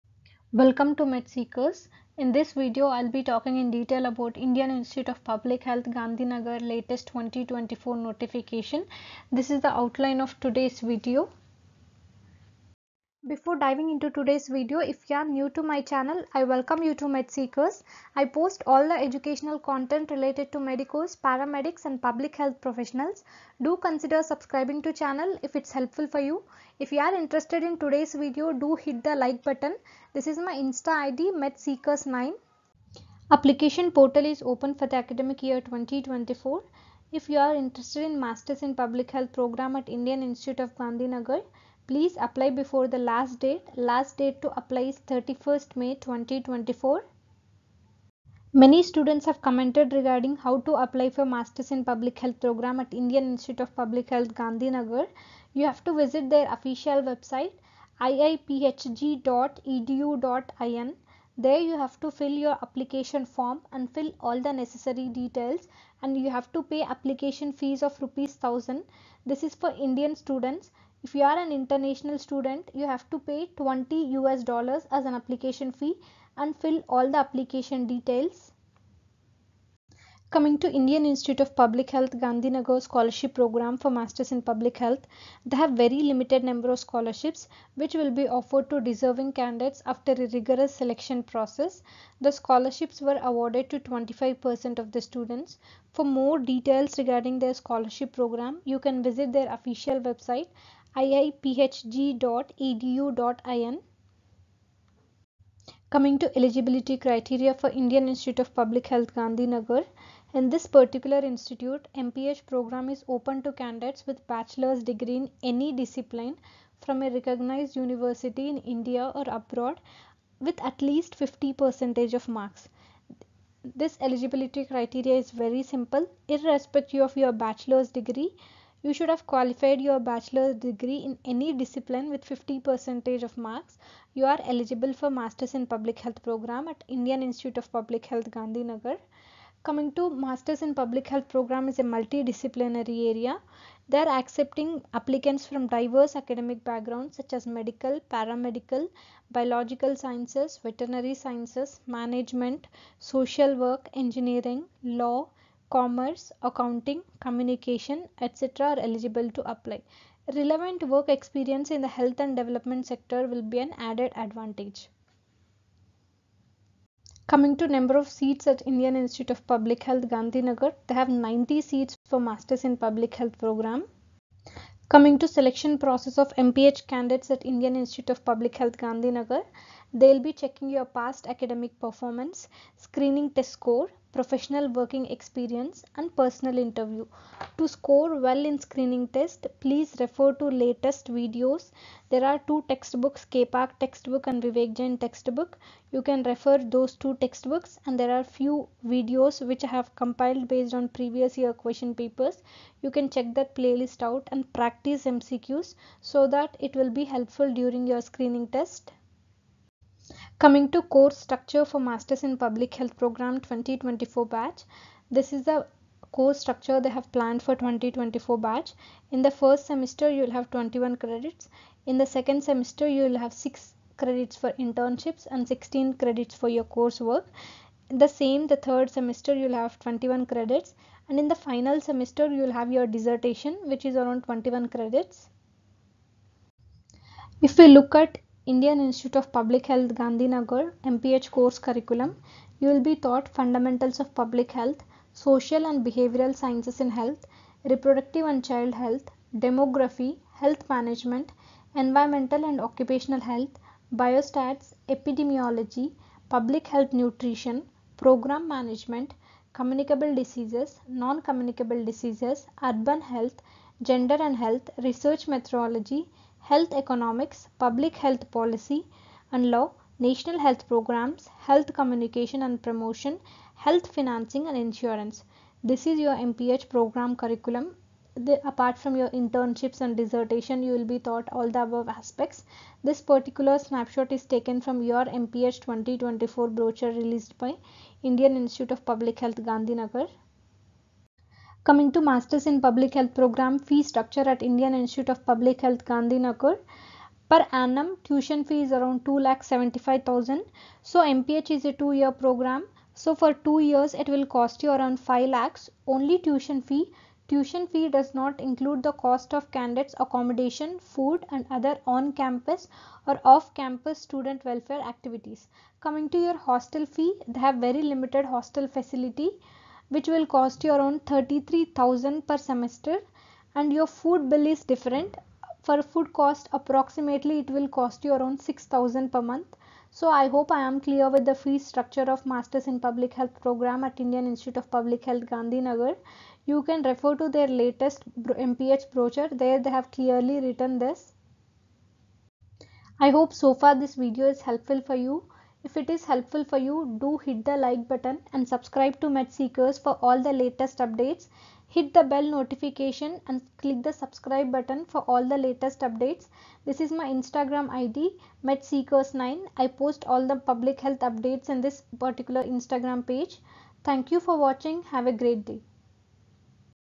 Indian Institute of Public Health MPH 2024|Completedetails MPH2024
Indian Institute of Public Health Gandhi Nagar Masters in Public Health Program 2024. This video contains all the Details Related to IIPHG  Masters in Public Health Program 2024.

Books to Refer for IIPHG MPH Entrance Exam
👉Text Book for Public Health
👉PSM INCLUDING BIOSTATISTICS